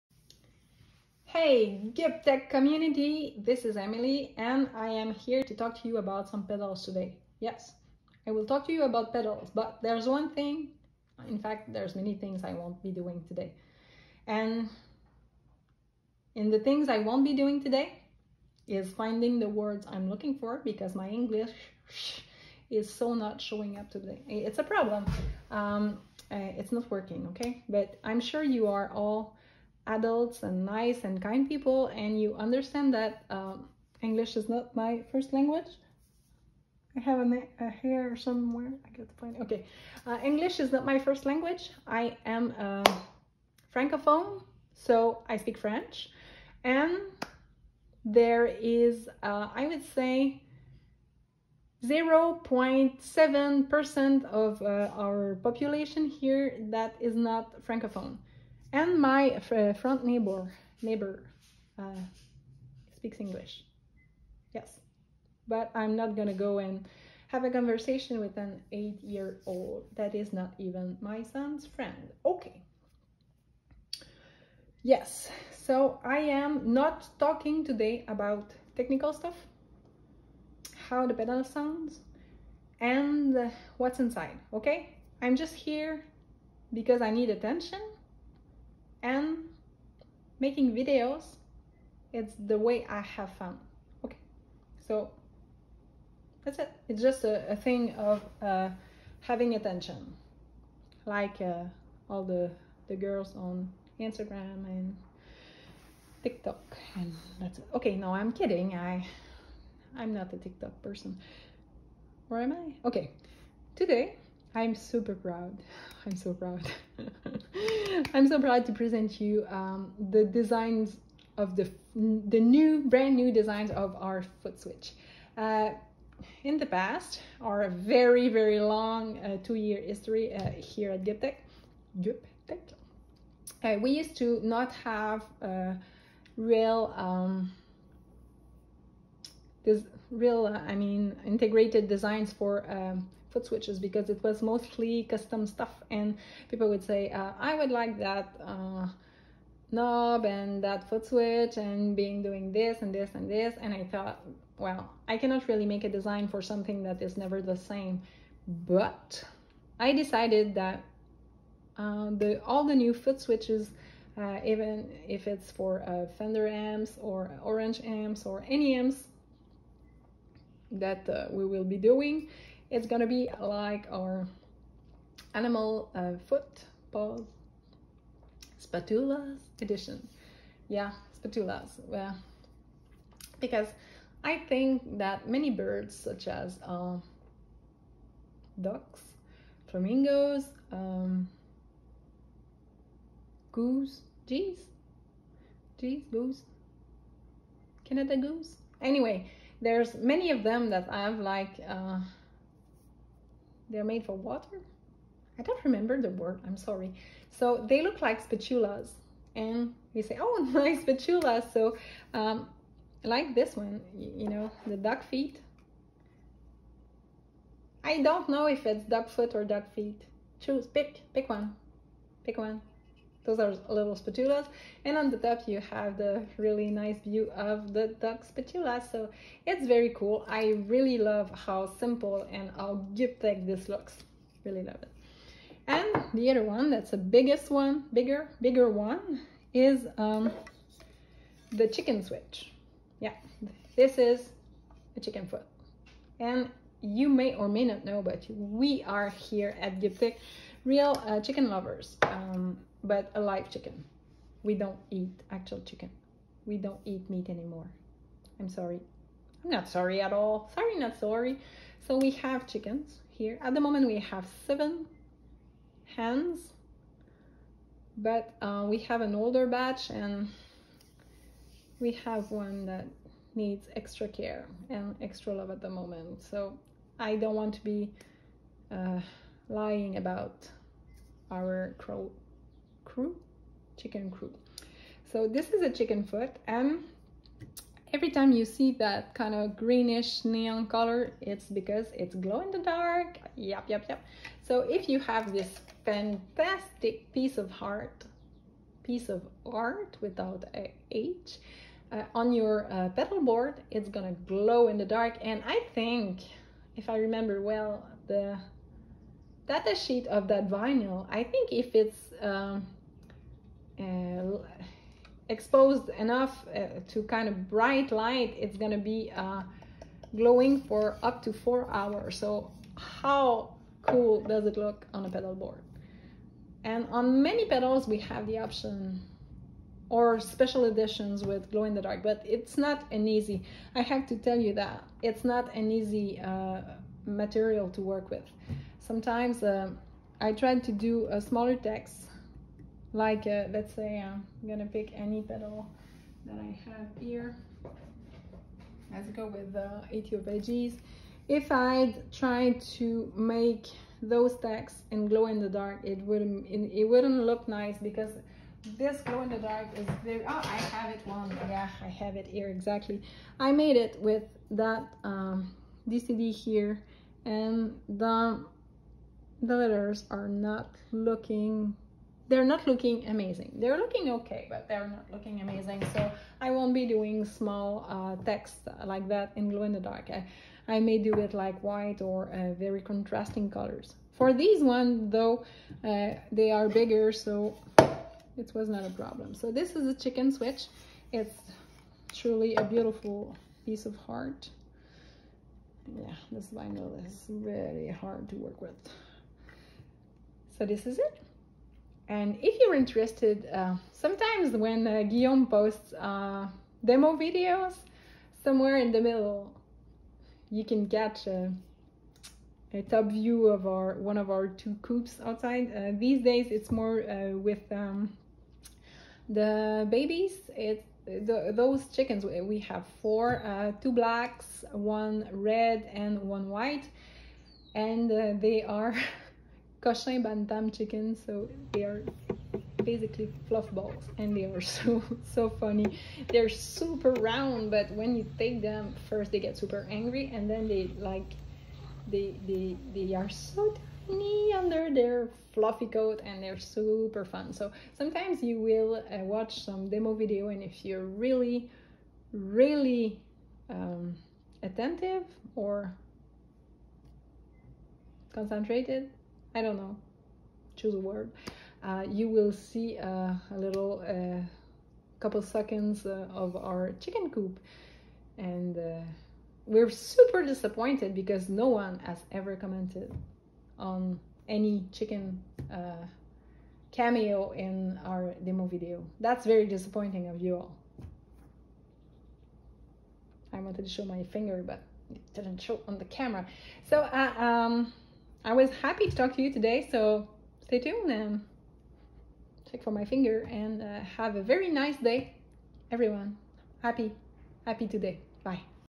GUP Tech Pedal Talk #23 - Amp Footswitch
Today we discuss two of our Amps footswitch.

The Duck Feet (For Mesa and Orange amp) and the Chkn Foot (for Engl amps).